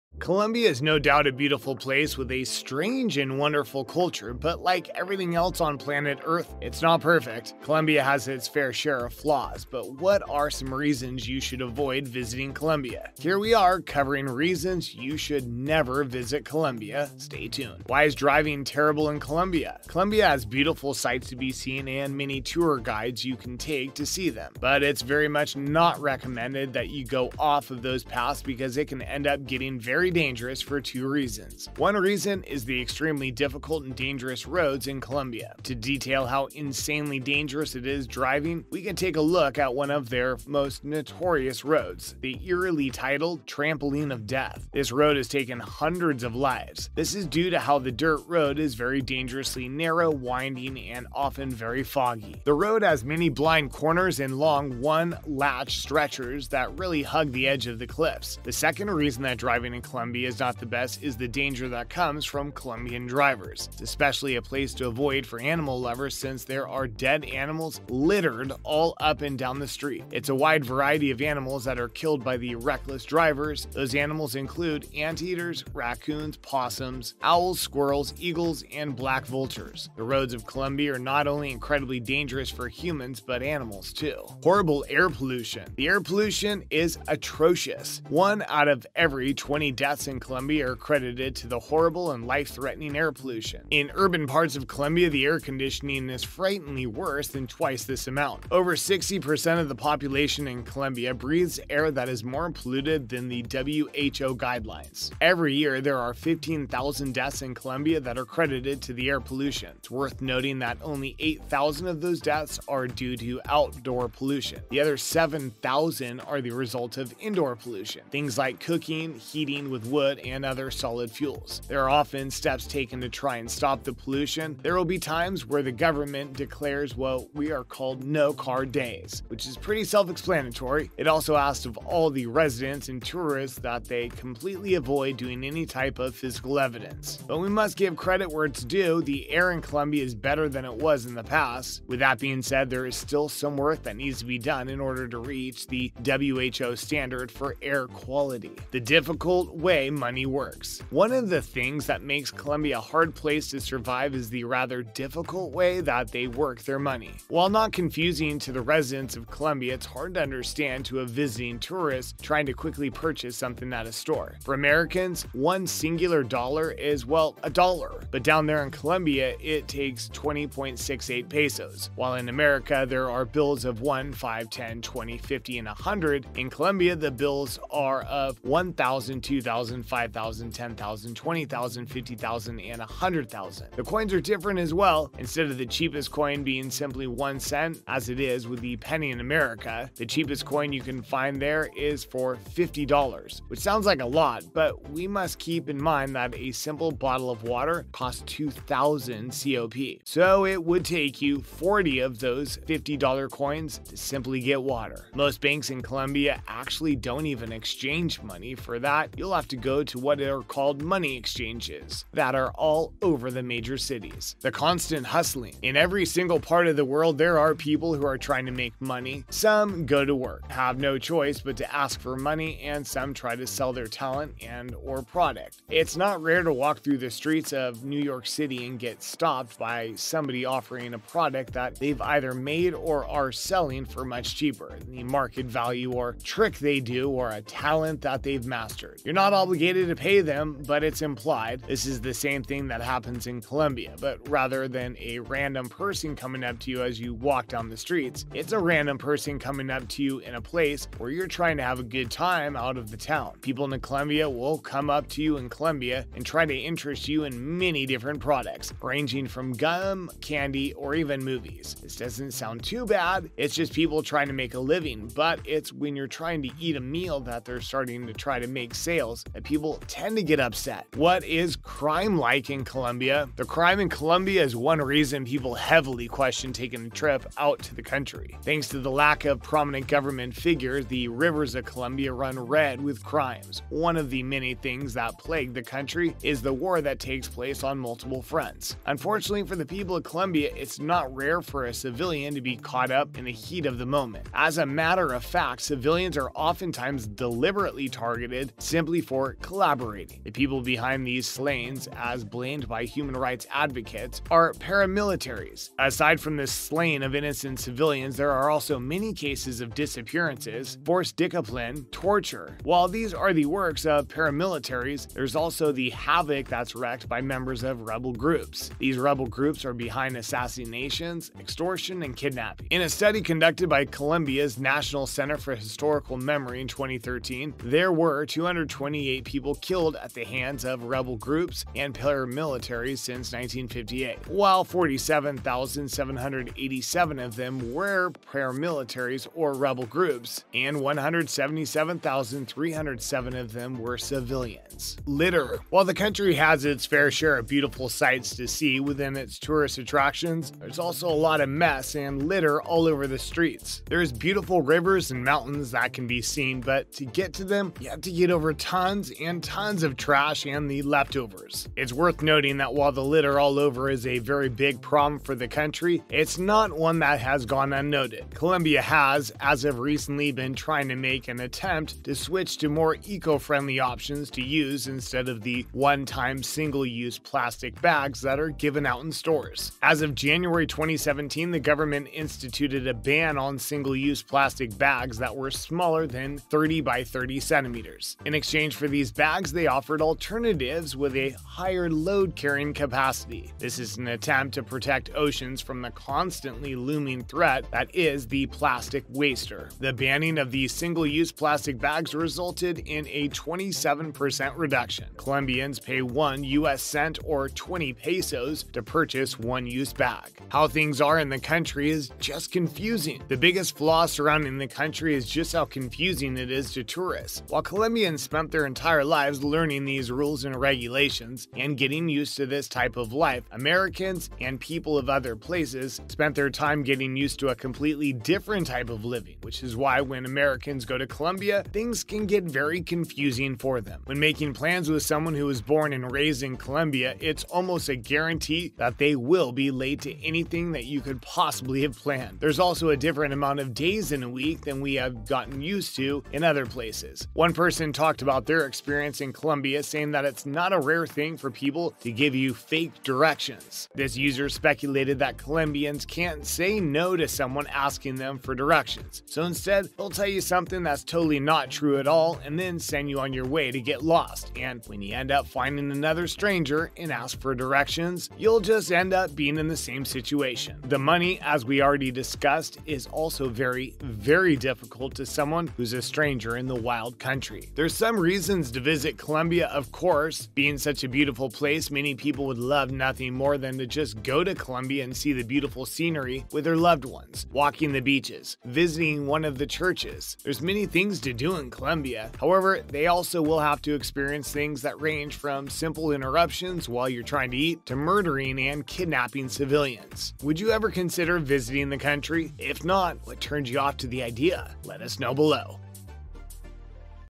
Reasons You Should NEVER Visit Colombia!
Welcome back to Travel Savvy.  Colombia is considered by many as an up and coming tourist destination. Thanks to its biodiversity and colorful festivals. But as they say, there’s always a positive and negative reaction to anything. Here are Reasons you should never visit Colombia.

One of the major concerns when visiting Colombia is the legality of cocaine. Cocaine is always a hot issue in Colombia, and if you casually talk about it while being there, you might find yourself in some good trouble. Stay in this video to know more Reasons you should never visit Colombia.